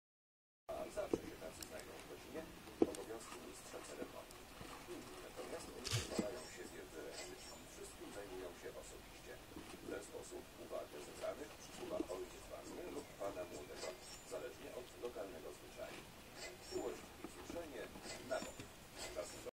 Girls with carrot :P
Yup... Eugenia brought piece of carrot up t the upper shed... She also bite few times... Then, Adolfina took it back to the ground floor of the cage. :P I think they like carrots and dried banana, same as well apples and peanuts. Time to time they eating the dried grass from the cage floor. :P:P